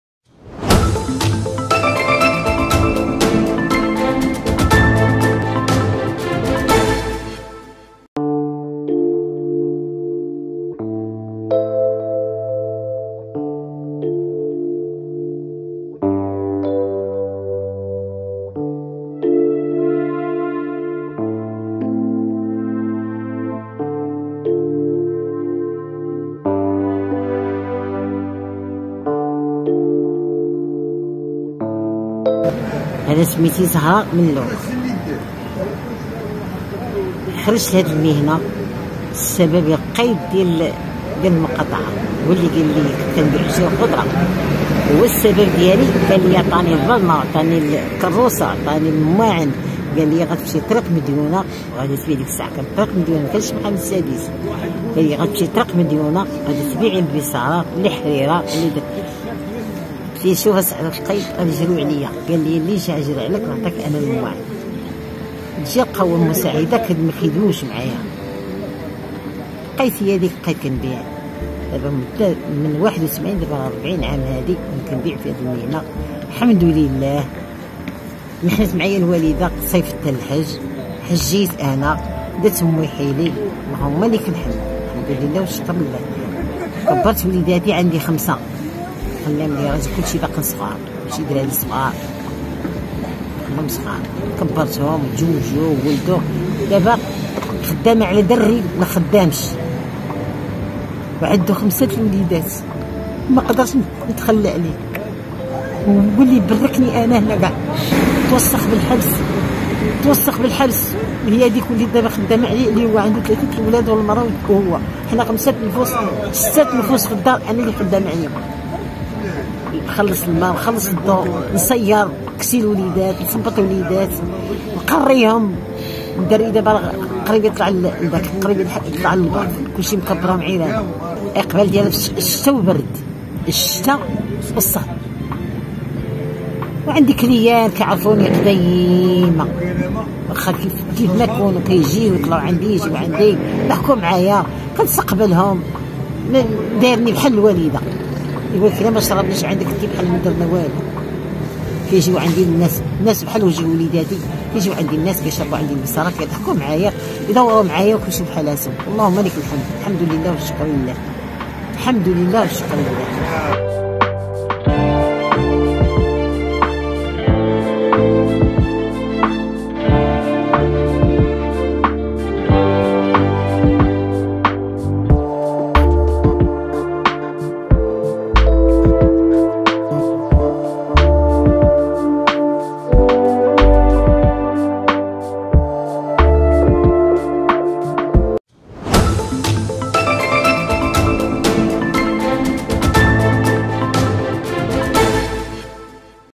مي زهراء.. أشهر بائعة بيصارة.. هذه قصة كفاحها
تببع البيصارة منذ 1981 بشارع مديونة.. مي زهراء أشهر بائعة بيصارة بالبيضاء.. هذه قصة كفاحها.

# أخبار_عاجلة